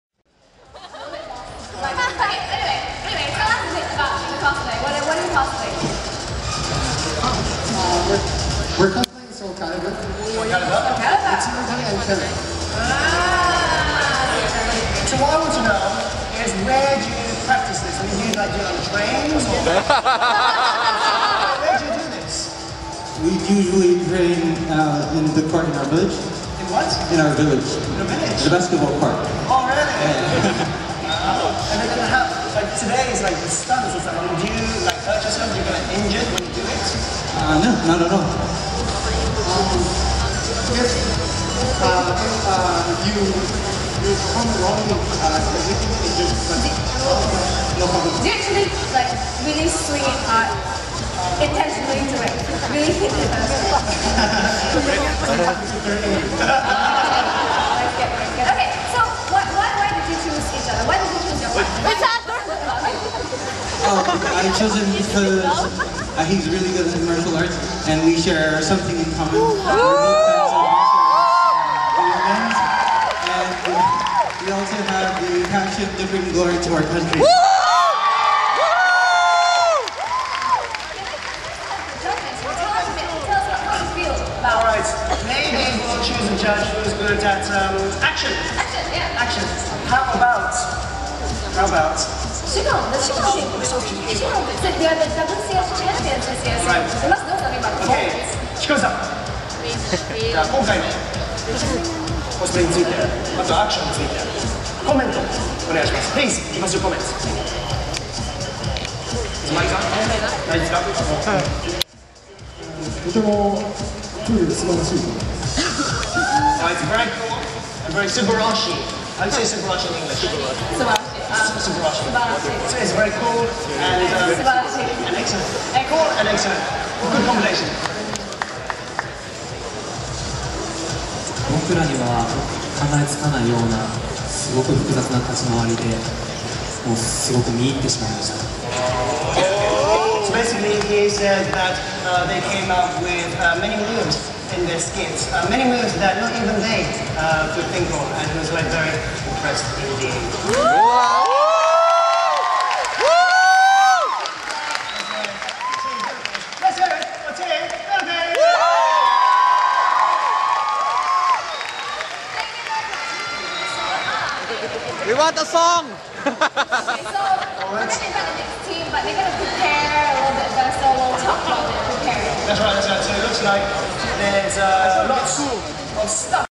AFA RCC Adventure Team Interview
Hosts interview the Philippine Adventure Team Marko Villaluz and Doho Lobaha Pururiin.